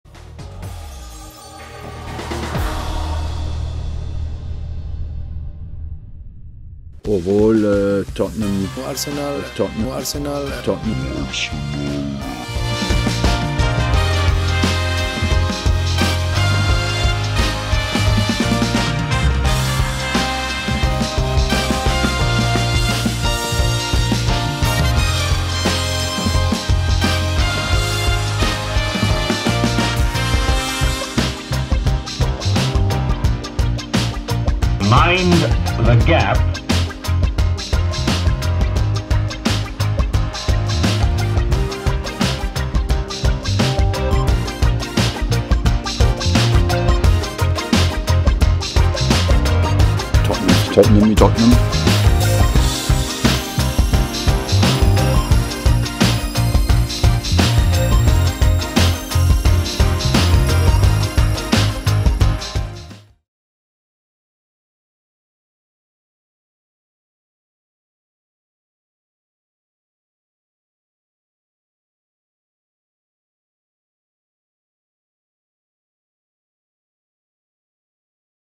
Arsenal v Tottenham - The North London Derby In Numbers
Arsenal host fierce local rivals Tottenham as both look to maintain their fight for the Premier League title.

Thanks